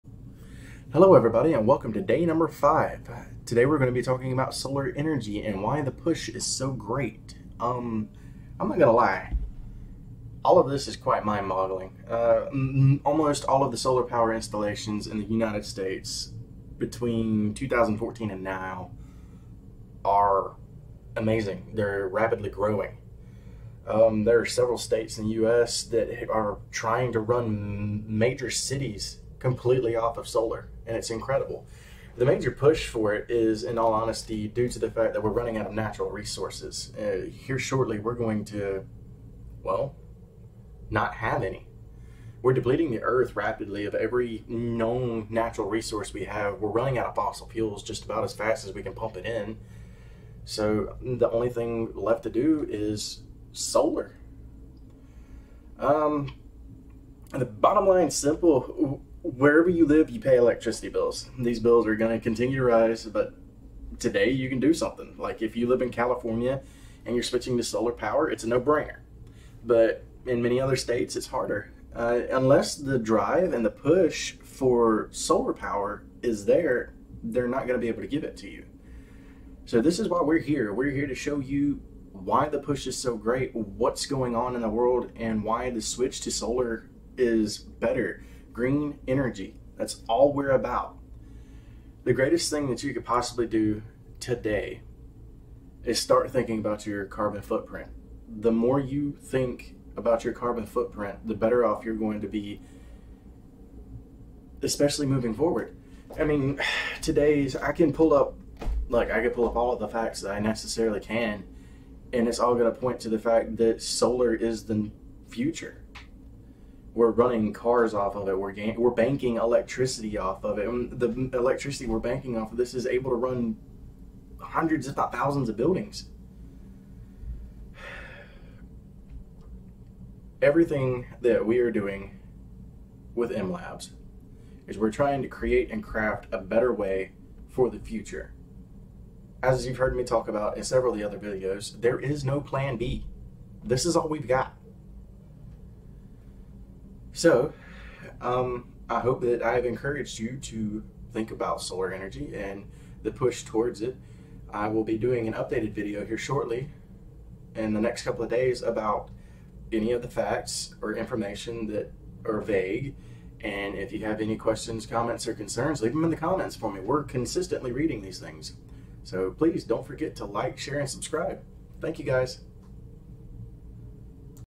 The Push for Solar Pt. 1
Todays topic is the push for solar and some small details on why its better to change.

Part 2 will arrive in 2 days.